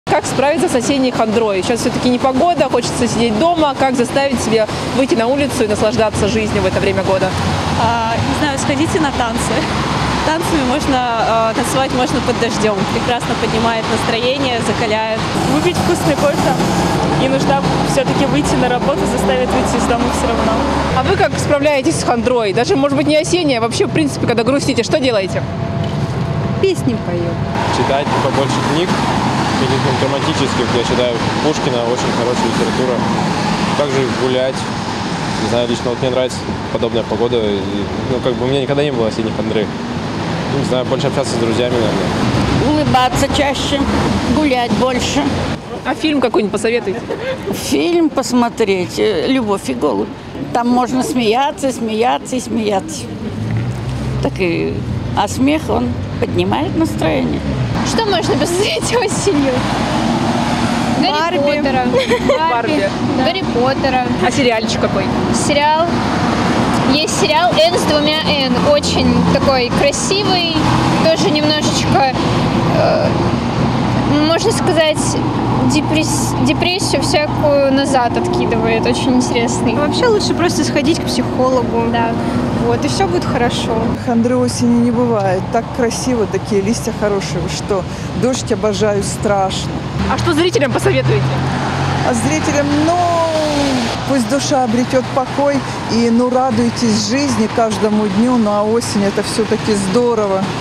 Опрос: барнаульцы поделились, как справиться с осенней хандрой
Пожелтевшие опавшие листья, дожди, люди в теплых пальто и с зонтиками в руках – такую картину можно сейчас наблюдать в Барнауле. В осеннюю погоду хочется остаться дома, укутаться в плед и бесконечно пить горячий шоколад. Но работу, обязательные дела никто не отменял.
Как наслаждаться жизнью, где черпать вдохновение, когда за окном непогода? Об этом съемочная группа телеканала "Толк" спросила жителей краевой столицы.